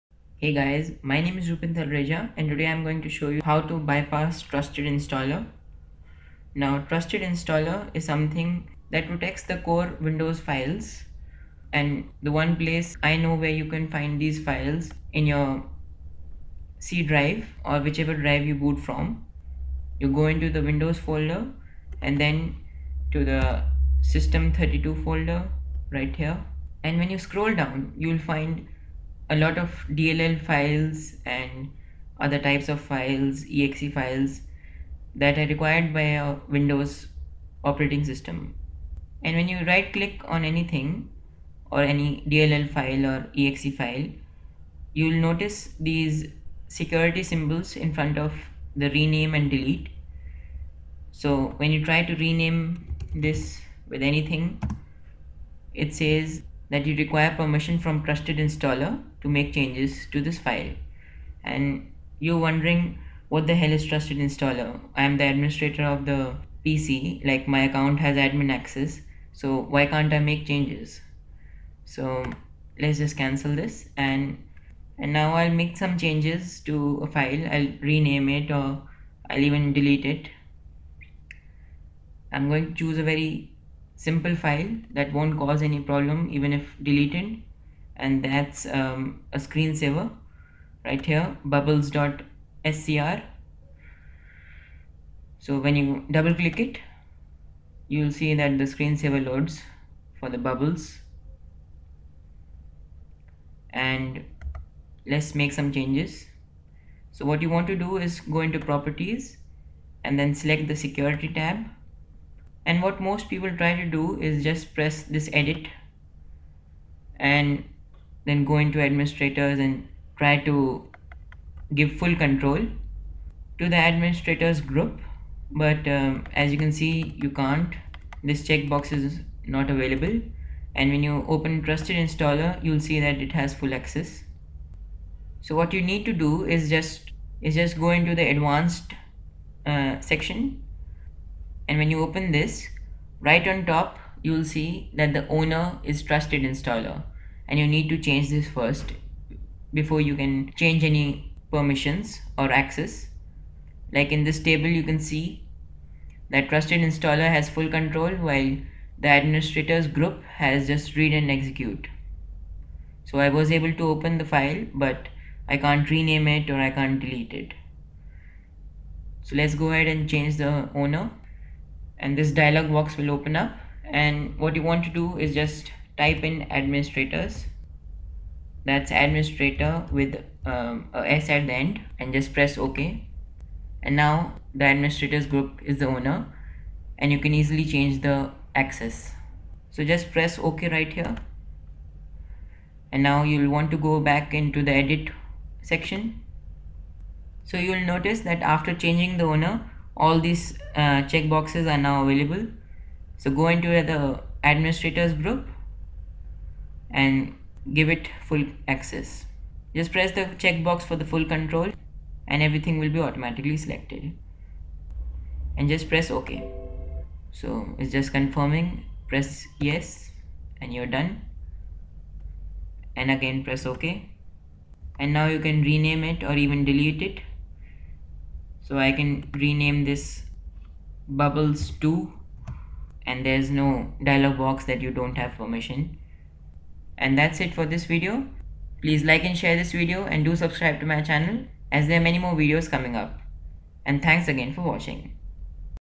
How to bypass TrustedInstaller
Taking back control of your computer and all its files
*WARNING* Taking ownership of Windows Apps folder and other System32 folders might break your windows, recommended that you backup the registry or create a system restore point.

If you are stuck in a situation where you took ownership of files and something breaks, the next windows update should fix it as well as if you restore/fix windows.